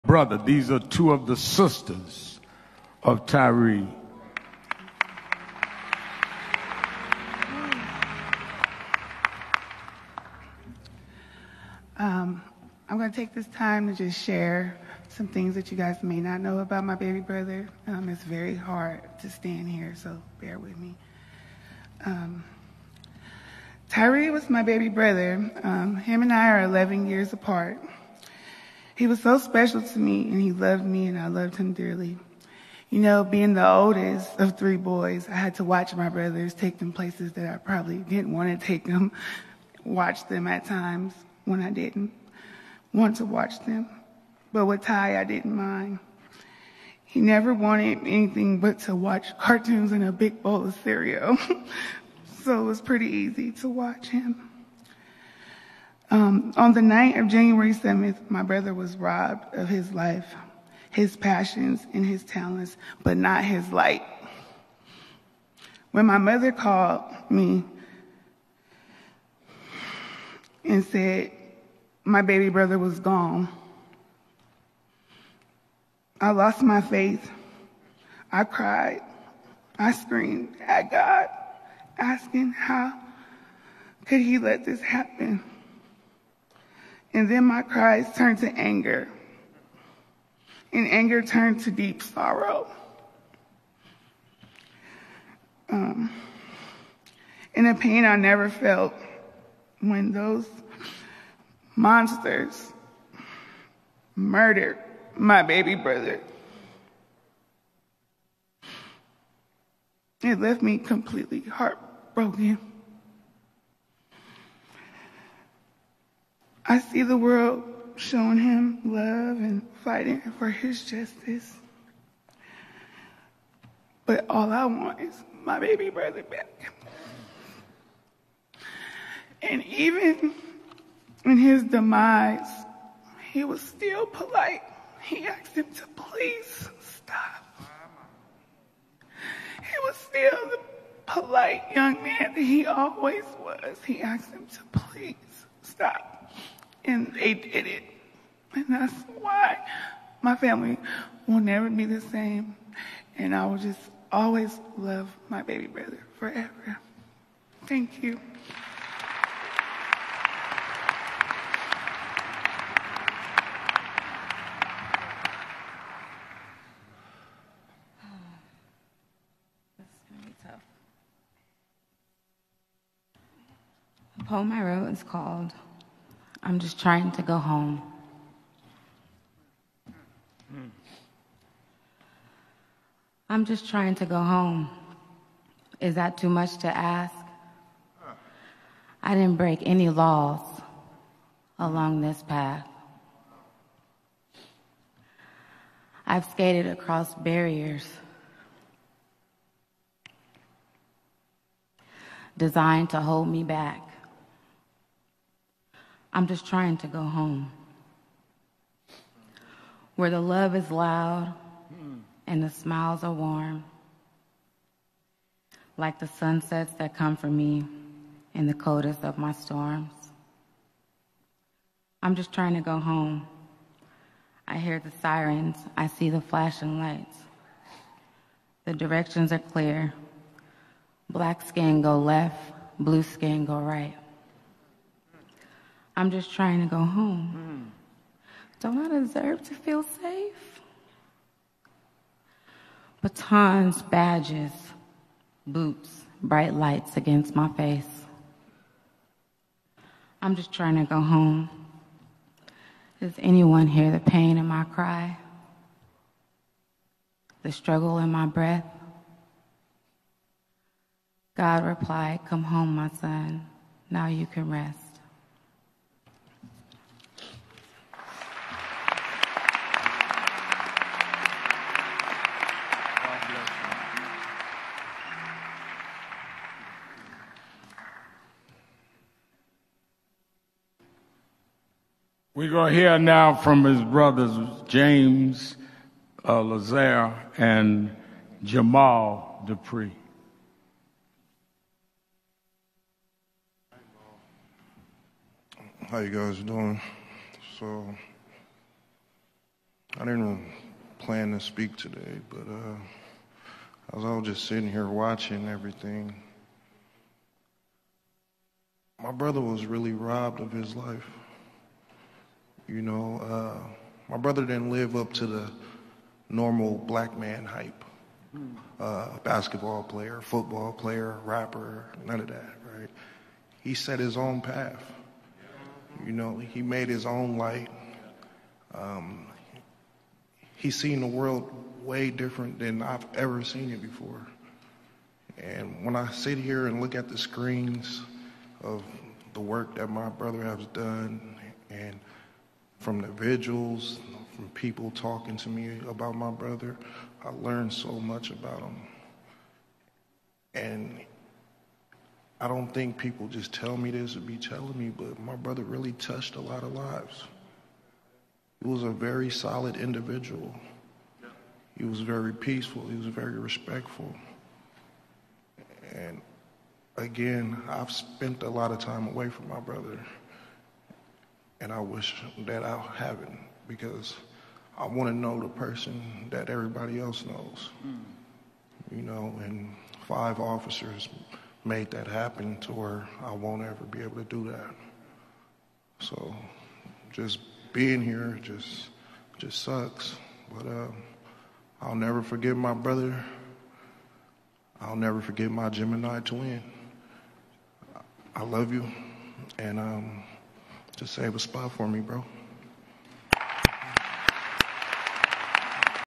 Bother, sisters of Tyre Nichols speak during his funeral service
The brother and sisters of Tyre Nichols spoke Wednesday during his funeral service.